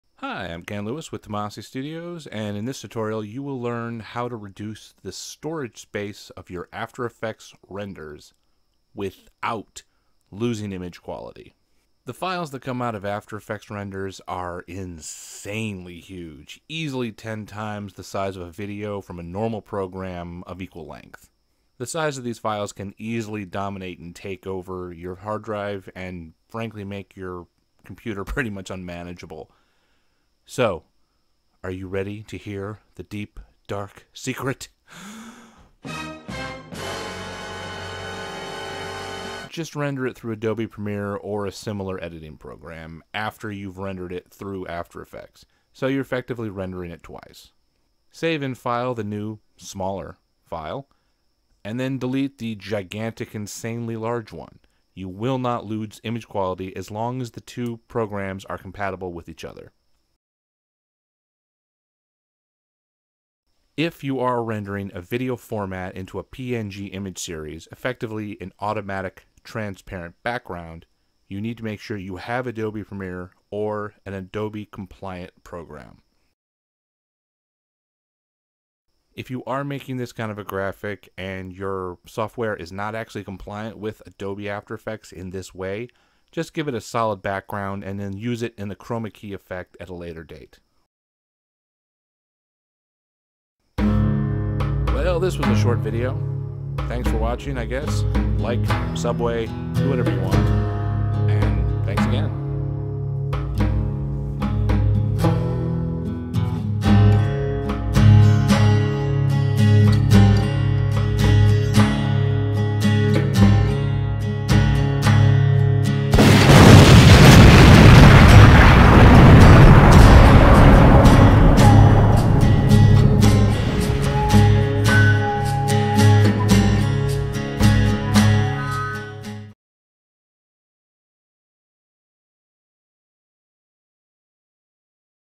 After Effect Tutorial How to Reduce File Size of Your Renders Without Losing Image Quality
After effects renders take up an insane amount of space on your hard drive. This tutorial will show you a how to make the files smaller without loosing quality.